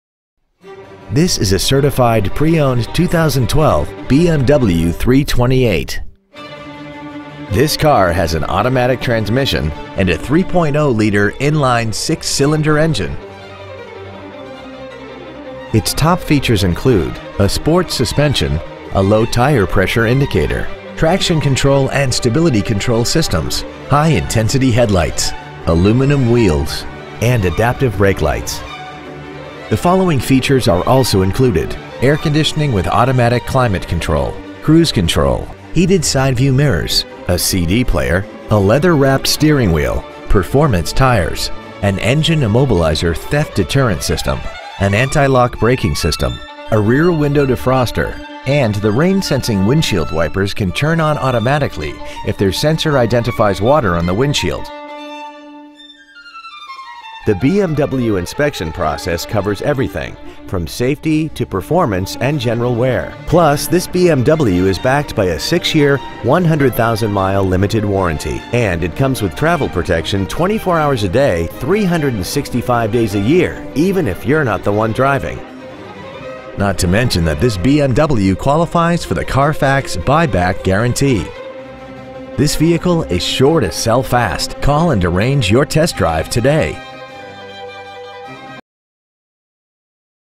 Used 2012 BMW 328i The Woodlands TX 77384
Year : 2012
 Make : BMW
 Model : 328i
 Engine : Gas I6 3.0L/183
 Trans . : Automatic
 Exterior : Jet Black
 Miles : 10
 Interior : Black